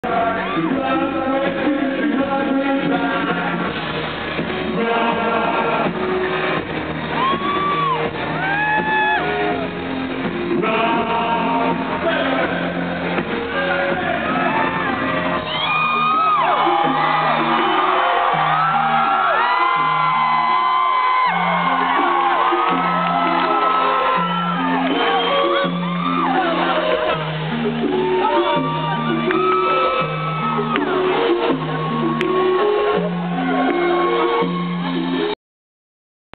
Depeche mode 8/17/09 3rd row!
i was in 3rd row but got to stand by the stage most of the show! AMAZING =D glad he looked so well =)